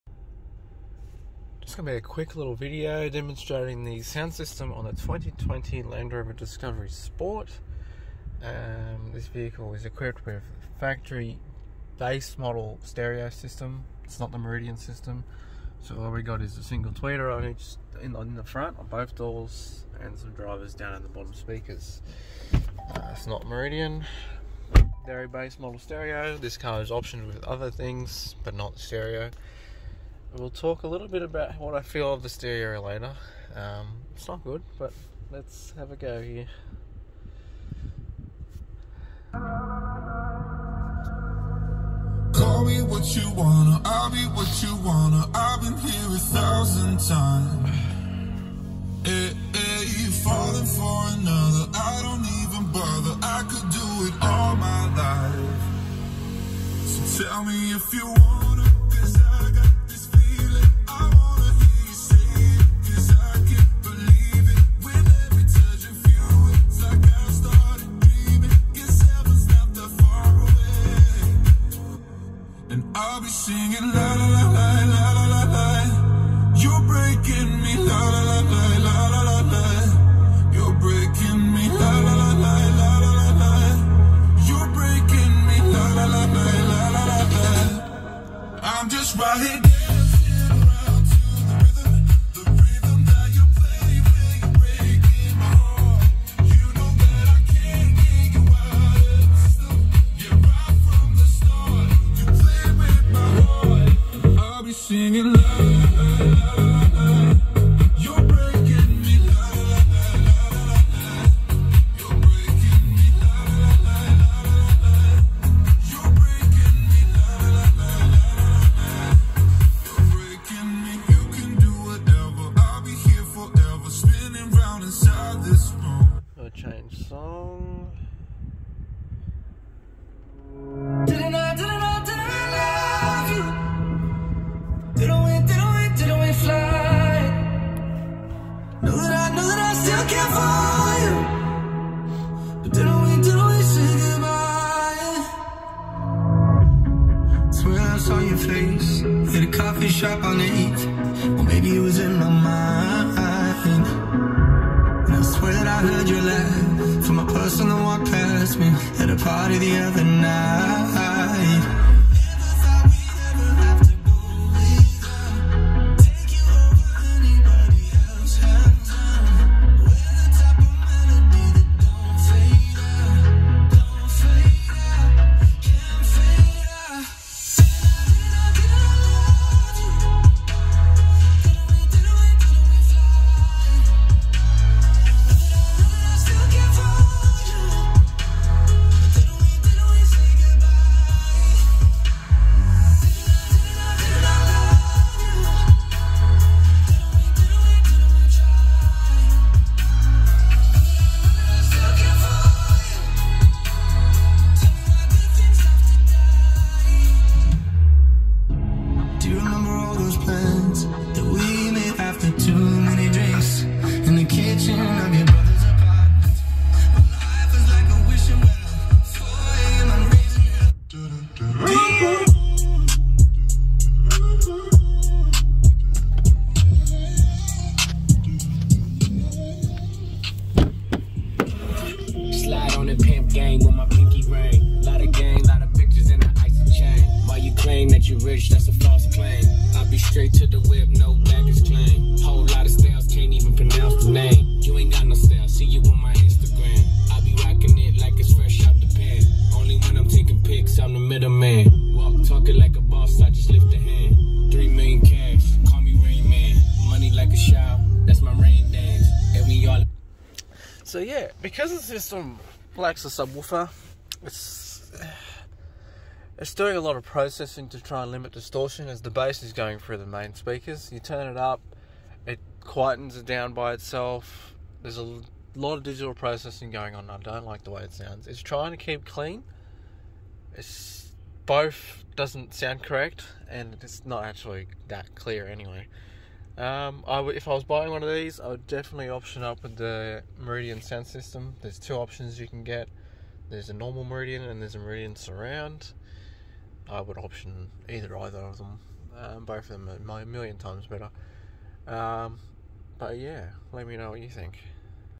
Land Rover Discovery sport 2020 sound system test
Just a quick demo of the base model stereo system that is a available on the 2020 discovery sport